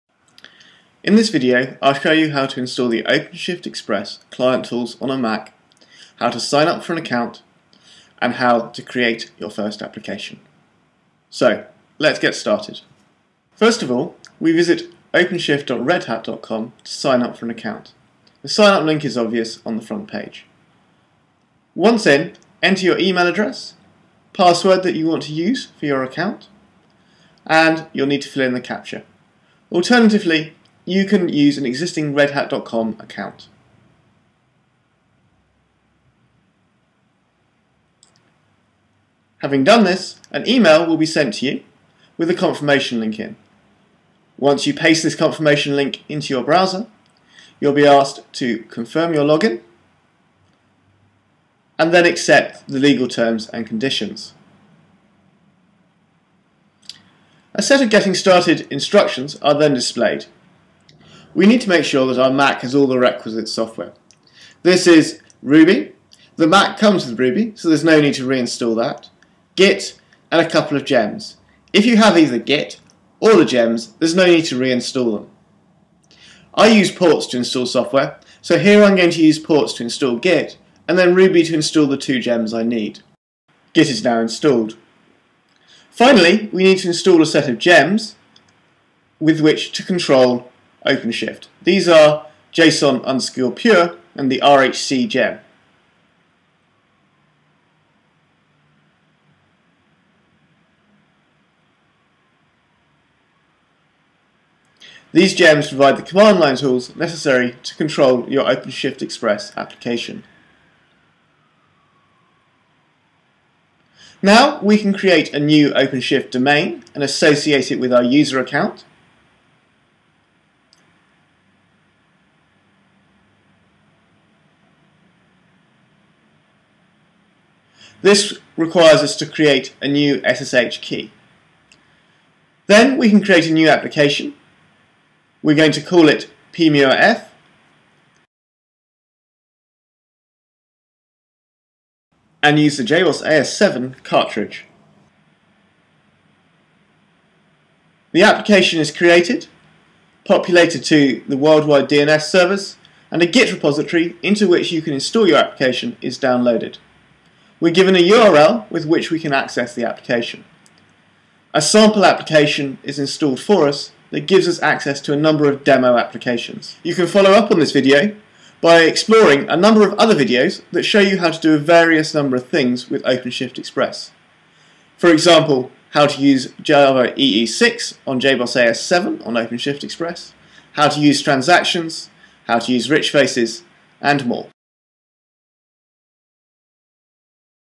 Intro to OpenShift Express (Mac)
This video demonstrates how to install the OpenShift Express client utilities on a Mac. Furthermore, it provides a walk through of signing up for an RHN account using the OpenShift website and details steps to allow a user to create their first express domain.

Please go to openshift.redhat.com for more details.